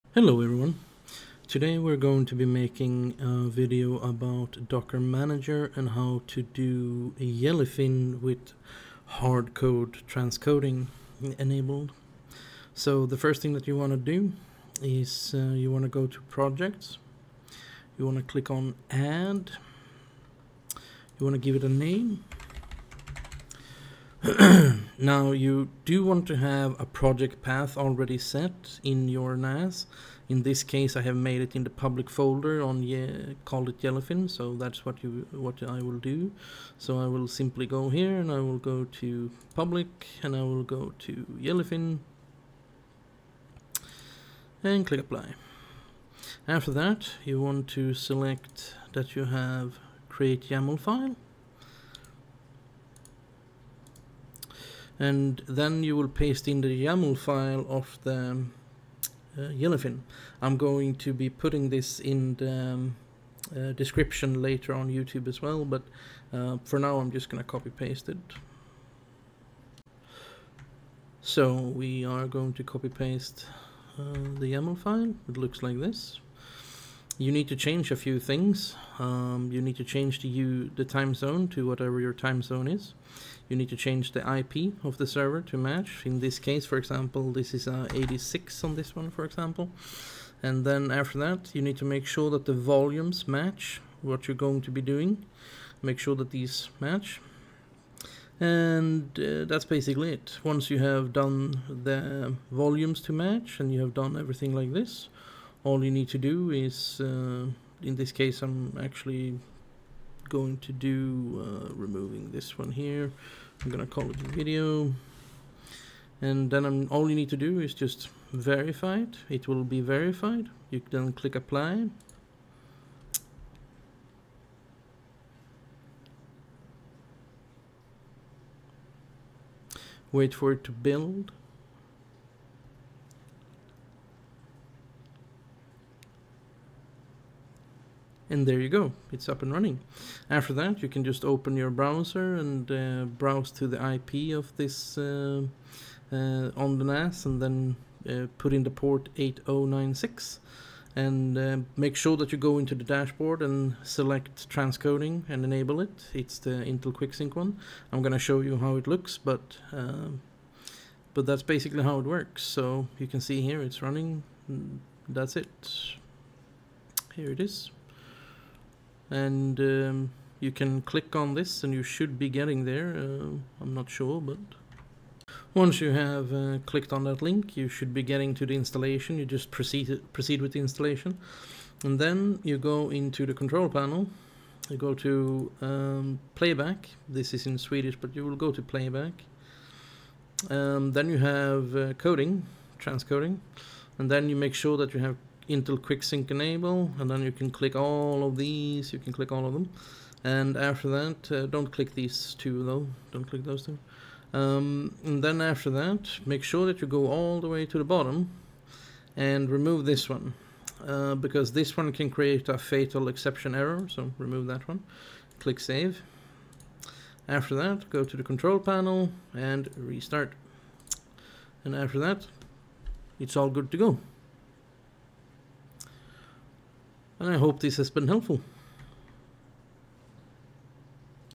TerraMaster Docker Manager Jellyfin hardware transcodeing enabled
Hello short video to update the previous video how to enable the transcodeing on jellyfin and setup jellyfin on terramaster nas system.

if you do not know how to find the Volumes and directory all you have to do is open file manager and right click and click property it will then say the location you need to enter in the yaml file :)

services:
  jellyfin:
    container_name: jellyfin
    environment:
      - PUID=0
      - PGID=0
      - TZ=UTC+01:00
      - DOCKER_MODS=linuxserver/mods:jellyfin-opencl-intel
    volumes:
      - /Volume1/public/jellyfin/config:/config
      - /Volume1/tvshows:/data/tvshows
      - /Volume1/movies:/data/movies
    devices:
      - /dev/dri:/dev/dri
    ports:
      - 8096:8096
      - 8920:8920
      - 7359:7359/udp
      - 1900:1900/udp
    restart: unless-stopped